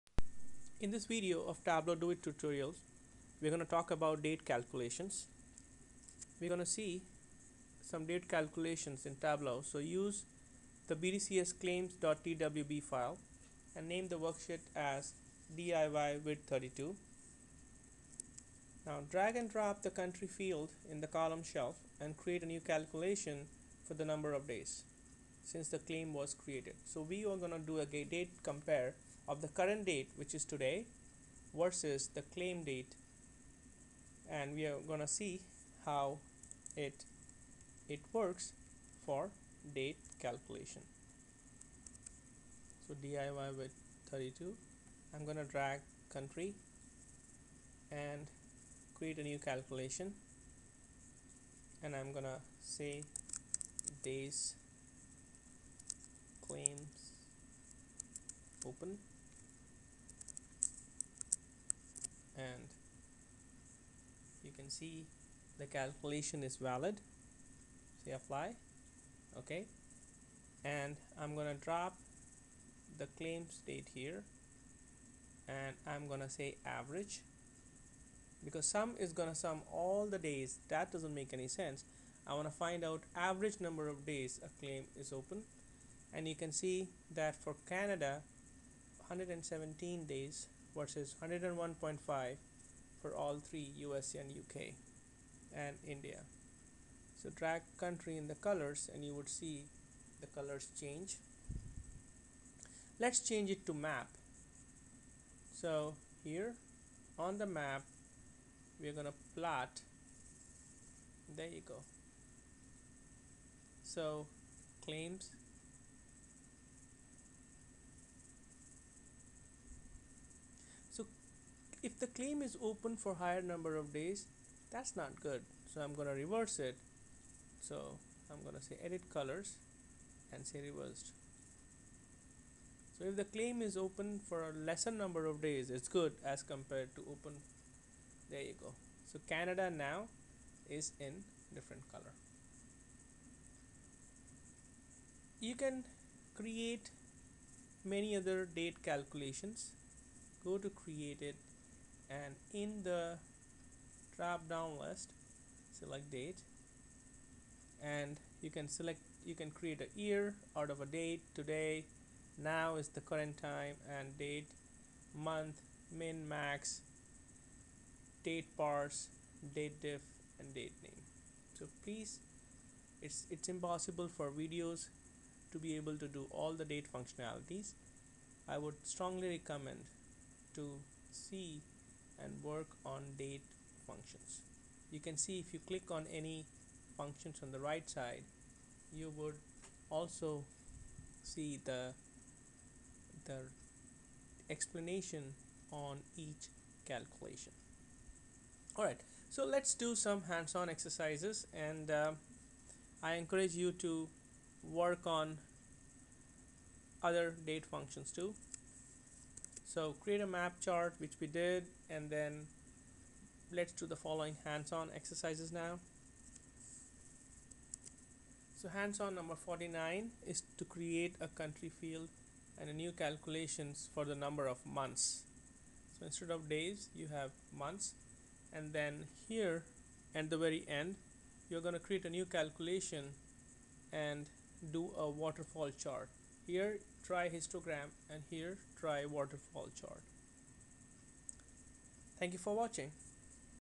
Tableau - Do it Yourself Tutorial - Date Calculations - DIY -32-of-50
Tableau - Do it Yourself(DIY) Tutorial - Date Calculations - DIY -32-of-50
by

In this video, we will see Date Calculations in Tableau
1- Open the TWB file used in video - 29 - BDCS Claims.TWB
2- Name the worksheet as DIY- Vid-32
3- Drag and drop the Country field on the Columns shelf.
4- Create a new calculation, for the number of days since the claim was created. DATEDIFF ('day',Claim Date, Today())
5- Create a map chart for Average of Claim Open Days.
6- Let's do the following hands-on exercise.

Hands on - DIY #49
1- Create a new Tableau Workbook.
2- Drag and drop the Country field on the Columns shelf.
3- Create a new calculation, for the number of Months since the claim was created.
4- Create a histogram for the Claim Open Months

Hands on - DIY #50
1- Create a new Tableau Workbook.
2- Drag and drop the Country field on the Columns shelf.
3- Create a new calculation Claim Open days - II, for the number of days since the claim was created. Don't reuse the calculations from the exercise above. Create a new one.
4- Create a Waterfall chart for the Claim Open days - II.